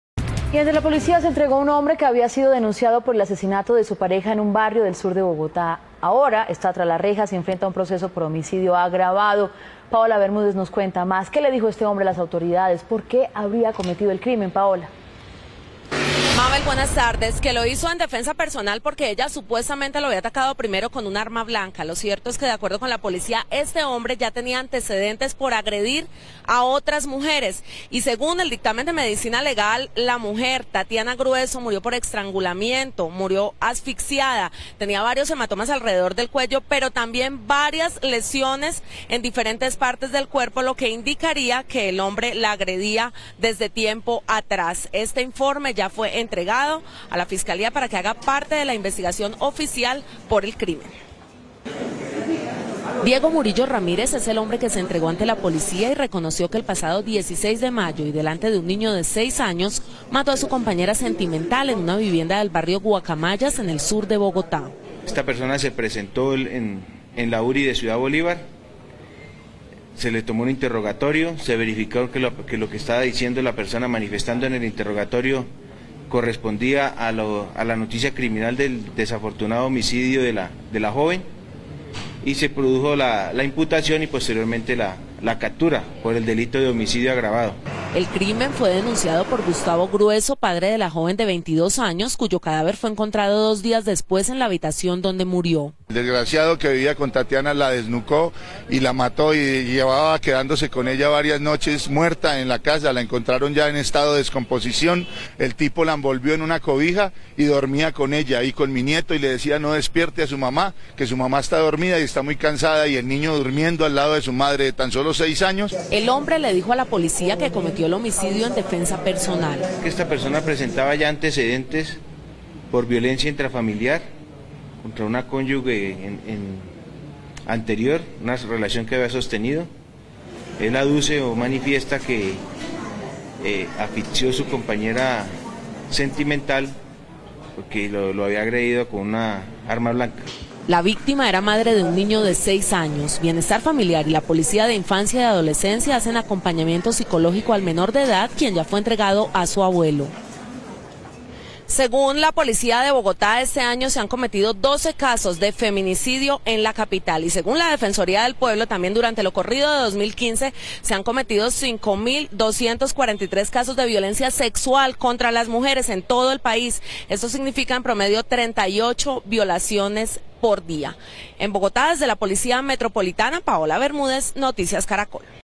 Se entregó hombre que asesinó a su pareja y escondió cuerpo
Emitido el 25 de Mayo de 2015. La Fiscalía dio detalles de este caso a través de un comunicado en su portal digital:"Una juez penal municipal de Bogotá, con función de control de garantías, cobijó con medida de aseguramiento en establecimiento carcelario a Diego Murillo Ramírez, a quien se le imputó el delito de homicidio agravado.La juez consideró que el imputado representa un peligro para la comunidad y ante la pena a imponer, en caso de ser hallado responsable, era necesaria la medida intramuros.Por su parte, el fiscal de la URI deTunjuelito indicó en audiencia pública que el imputado, quien aceptó los cargos, fue quien el pasado 16 de mayo, dentro de una residencia del barrio Guacamayas, en la localidad de San Cristóbal, al sur oriente de Bogotá, asesinó a su pareja sentimental, Tatiana Andrea Grueso Calderón de 22 años.Según la investigación, los hechos se presentaron luego de una discusión entre la pareja, razón por la que el imputado la asfixió con sus manos.Tras el crimen abandonó el lugar, presentándosedías más tarde ante las autoridades del municipio de Chicoral, en el Tolima, donde al no existir ninguna orden de capturaen su contra continuó en libertad.Posteriormente, la Fiscalía citó a Murillo Ramírez, quien se presentó voluntariamente para ser judicializado.La pareja llevaba solo seis meses de vida marital, según se conoció en la audiencia, donde se expresó que la joven víctima dejó una hija de 6 años, la cual quedó bajo la custodia del Bienestar Familiar".